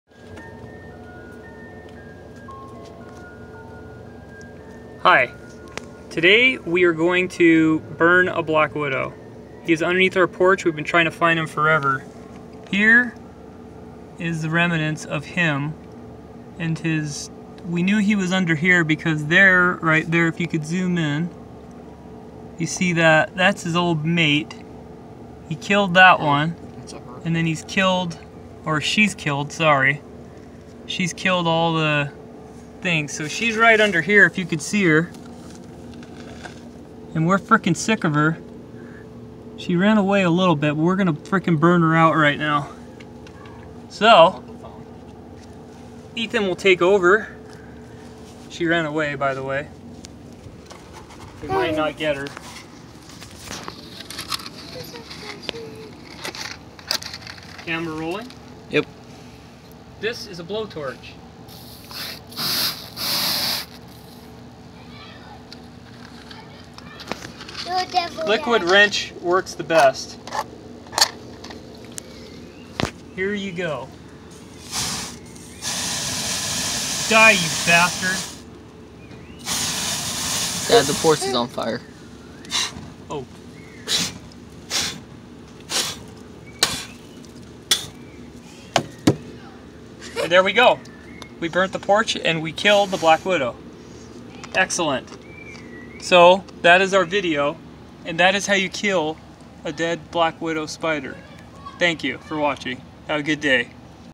Black Widow
Blow Torching a pesky black widow under the porch. Dude catches house on fire with liquid wrench.